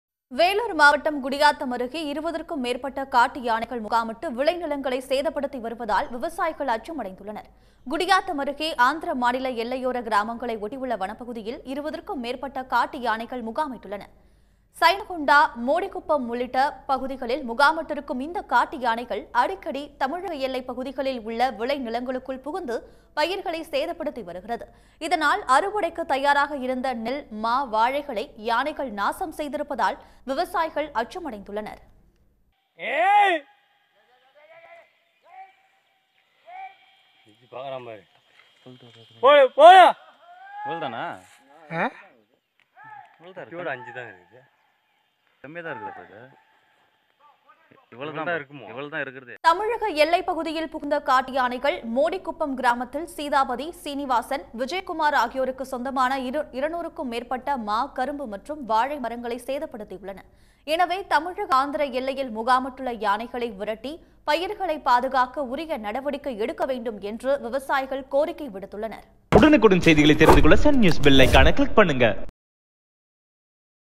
குடியாத்தம் அருகே காட்டு யானைகள் முகாம்; விவசாயிகள் கவலை | Kudiyatham Elephants
Kudiyatham | KudiyathamElephants | ElephantsCamp | குடியாத்தம் அருகே காட்டு யானைகள் முகாம்; விவசாயிகள் கவலை | Kudiyatham Elephants